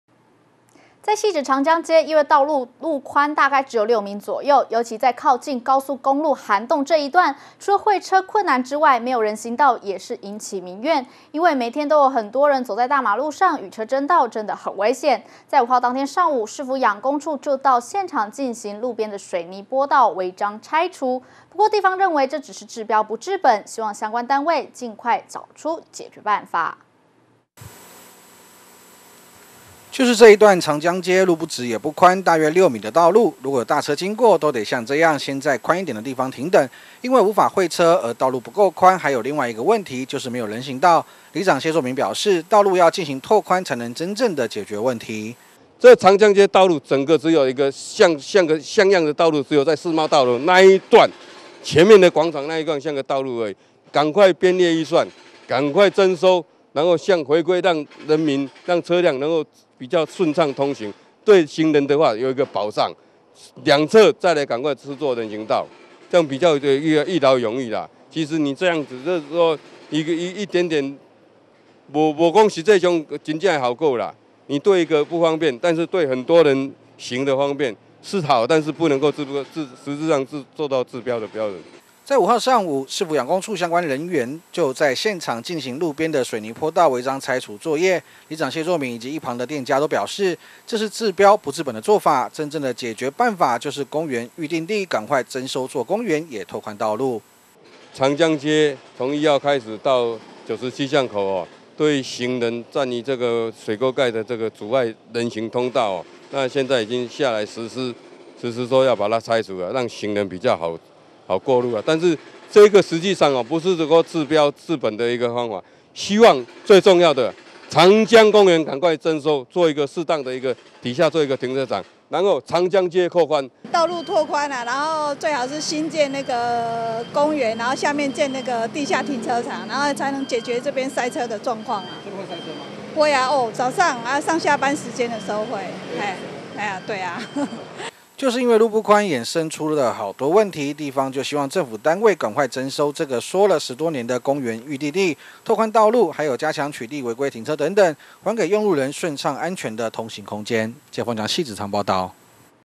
1040605觀天下新聞HD02 汐止區長江路窄難行 市府拆坡道民眾不滿
在汐止長江街，因為道路寬度大概只有六米左右，尤其在靠近高速公路涵洞這一段，除了會車困難之外，沒有人行道也引起民怨，因為，每天都只能走在大馬路上與車爭道，真的很危險，在5日上午，市府養工處就到現場進行路邊的水泥坡道違章拆除，不過地方認為這樣只是治標不治本，希望相關單位儘快找出解決辦法。